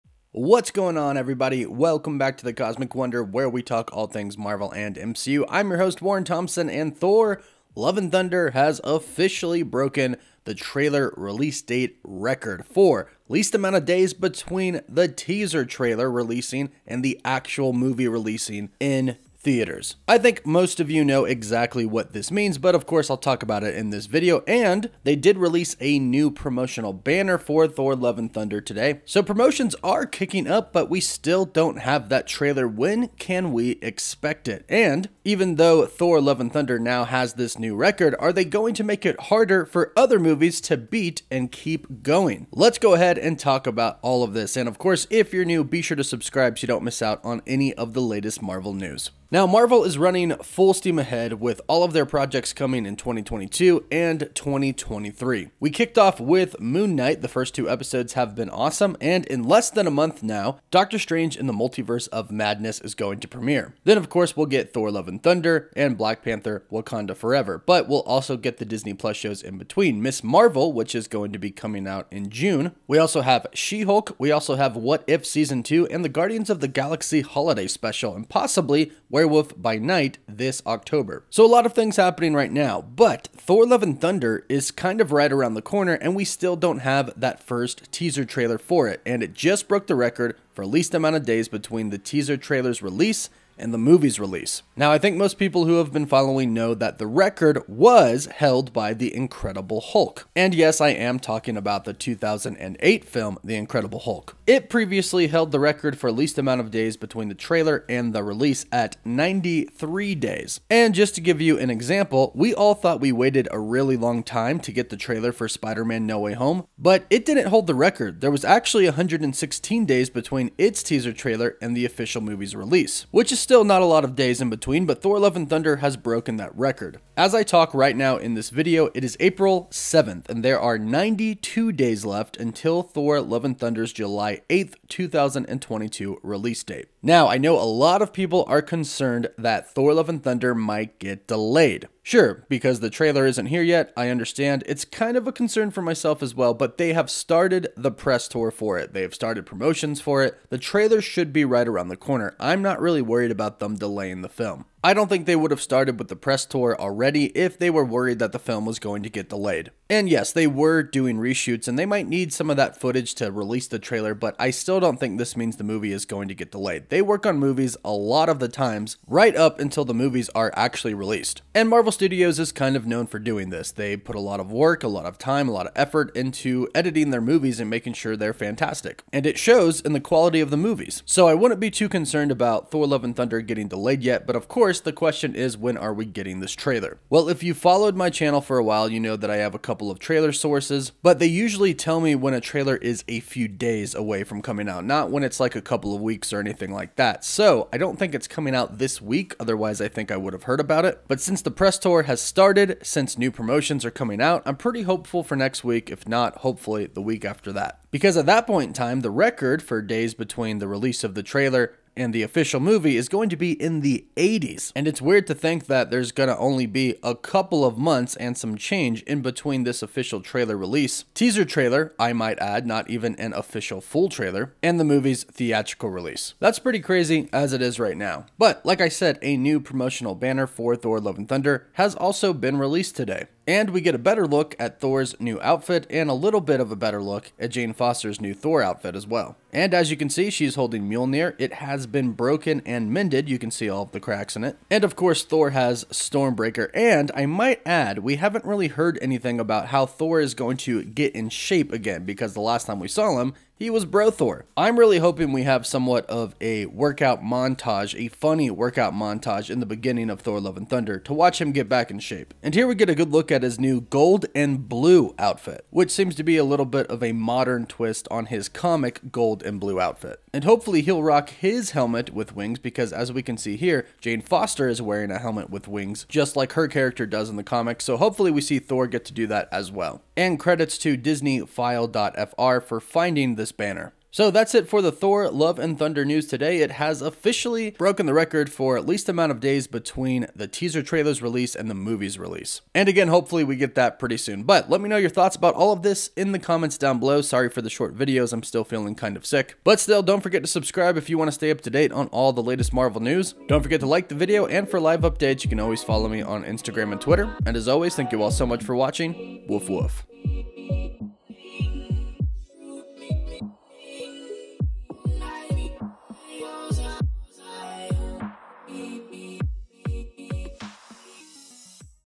Thor Love & Thunder Trailer Release Date BREAKS MARVEL RECORD! New Promo Look!
The Thor Love and Thunder teaser trailer has now broken a Marvel Studios record. Thor Love and Thunder now has the least amount of days between the teaser trailer's release and the movies theatrical release than any other MCU movie. Thor Love and Thunder teaser trailer should be coming soon as we have new promo's for Thor 4 and the press tour has begun. Thor Love and Thunder teaser trailer release date should be sometime within the next couple of weeks (hopefully)!